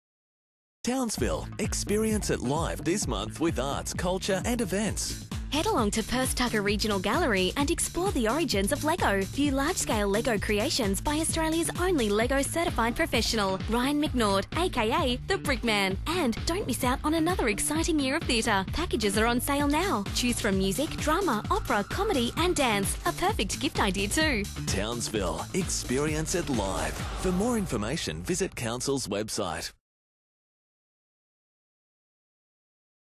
Experience it Live December - January
Experience it live this month in arts, culture and events.

Check out the largest ever exhibition of LEGO® history and products in Townsville at Perc Tucker Regional Gallery from 13 Dec-23 February.

2014 Theatre Season packages are now on sale. Choose from music, drama, opera, comedy and dance. There's a show to suit every taste. Book now at TicketShop.